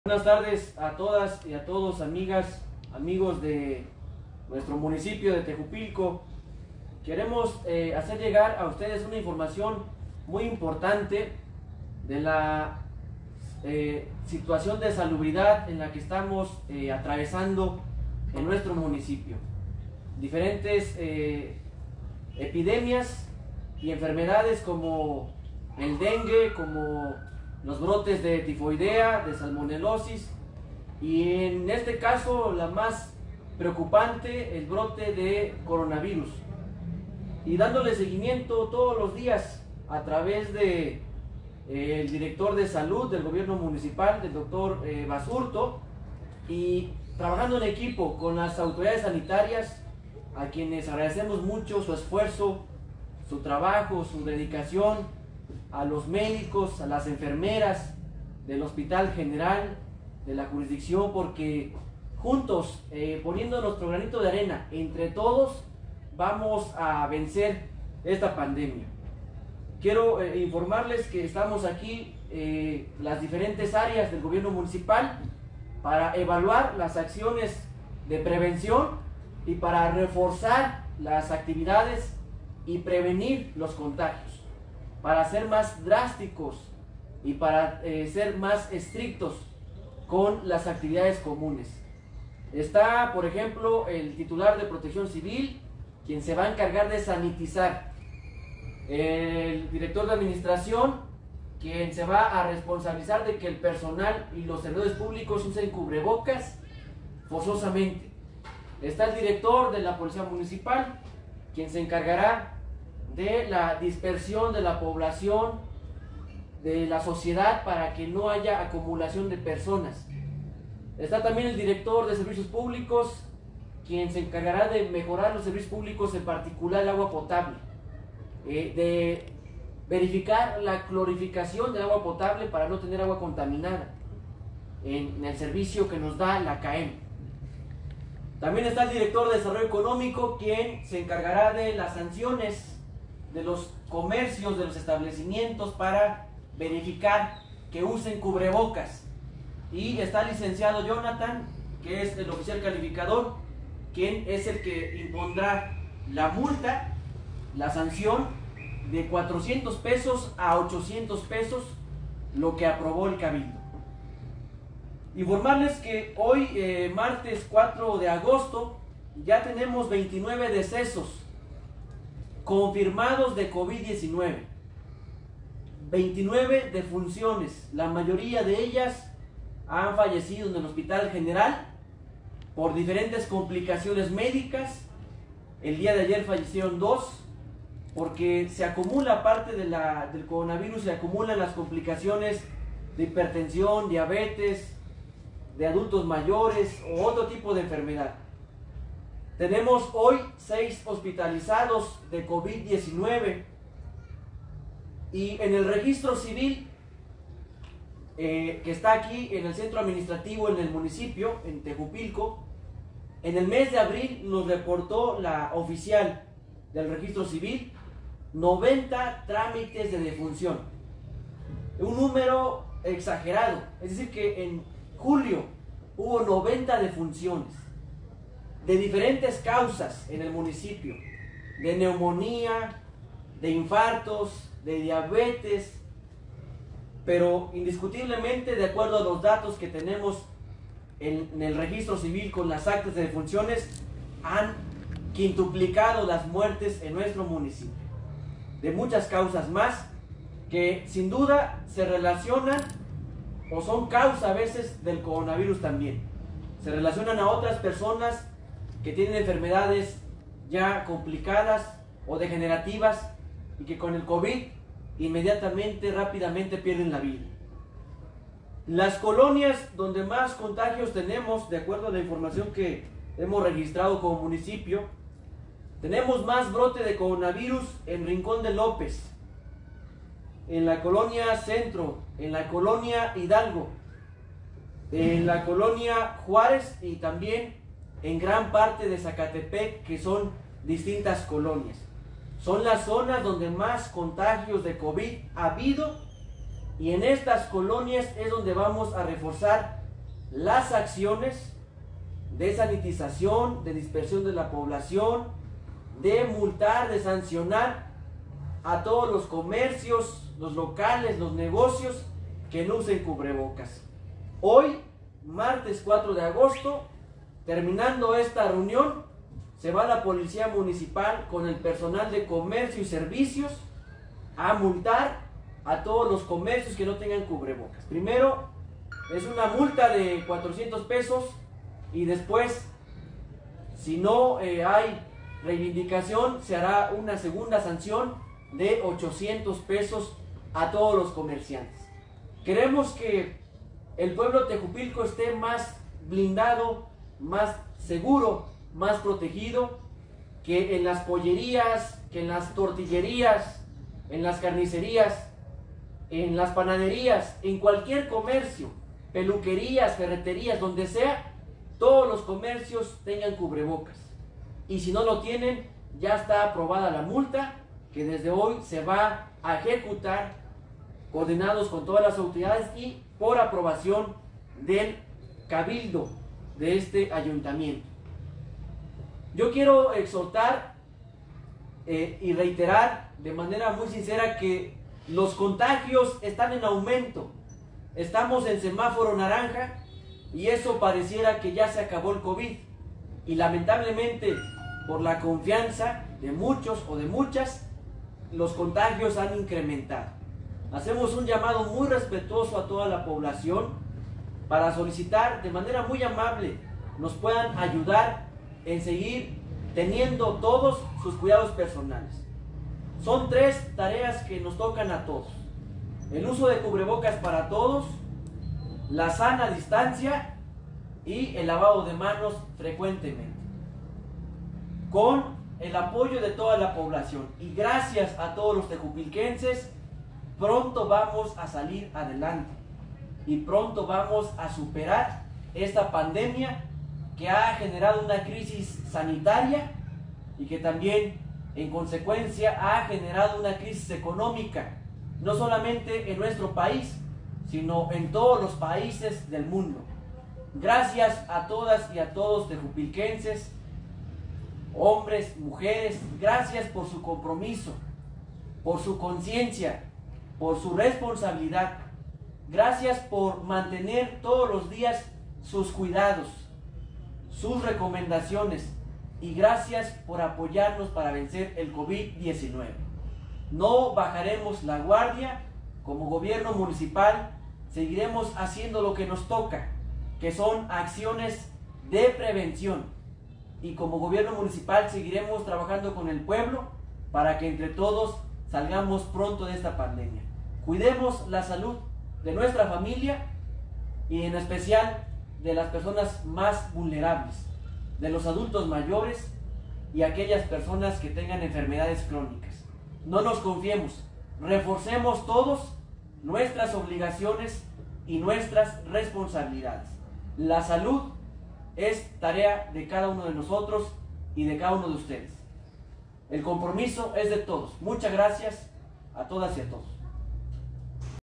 REPORTE COVID TEJUPILCO 4 AGOSTO 2020
El presidente municipal de Tejupilco, Manuel Anthony Domínguez Vargas, da a conocer que “hasta el momento ya tenemos 29 decesos, confirmados de Covid-19, existen 6 hospitalizados. las colonias con más brotes de Coronavirus, son Rincón de López, Colonia Centro, Hidalgo, Juárez y gran parte de Zacatepec; reforzaremos mucho más las acciones, de sanitización y dispersión de la población; así como la aplicación de sanciones a comercios, que no usen cubrebocas”.